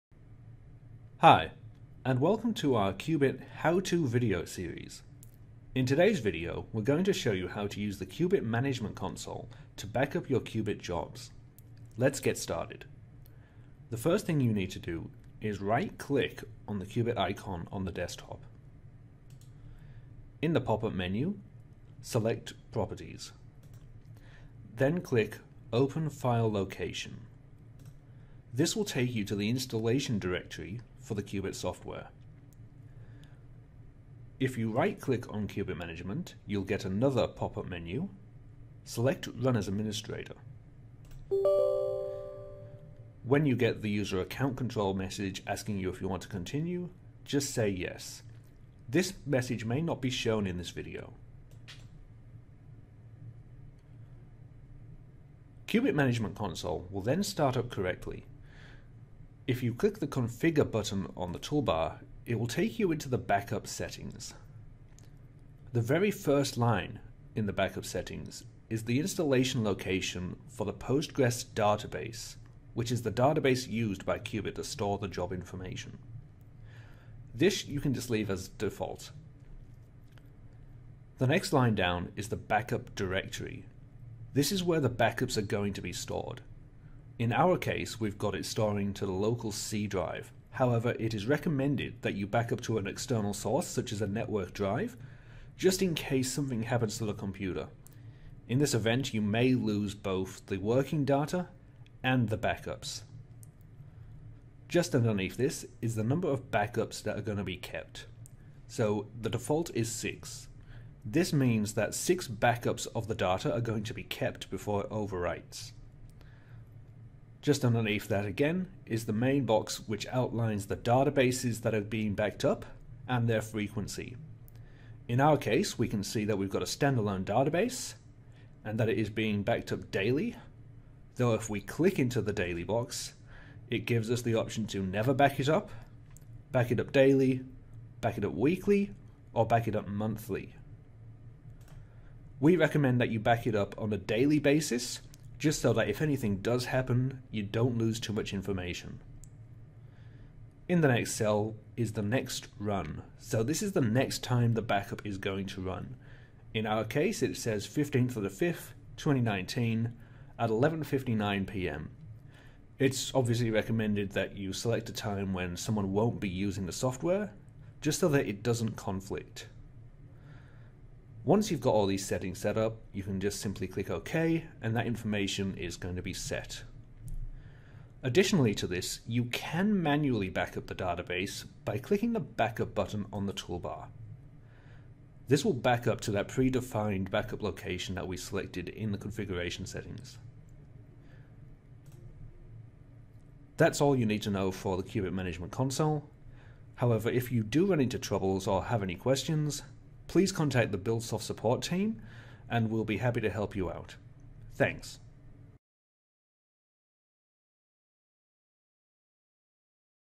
How to backup your data in Cubit
Learn how to backup your jobs in Cubit with this short tutorial. Performing your estimating and takeoff is simple with Cubit. This tutorial runs you how to backup your data to help you get the most out of the software.

Developed by Buildsoft, Cubit is industry leading estimating software designed to help you improve the efficiency, speed, and accuracy of your construction estimating.